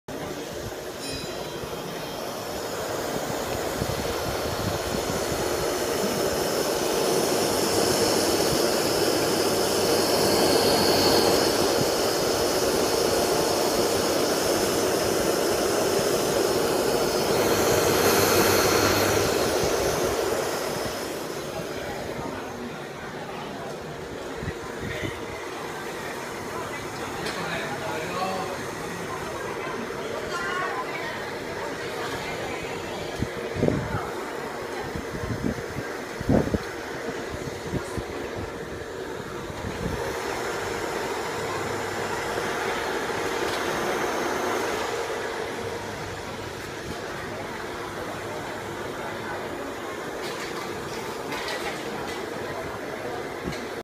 ESPA brand water pump with filter and cooling fan
This is at a seafood restaurant. Not sure where it leads to and from, but it might have something to do with fish tanks. This pump appears to be a centrifugal unit.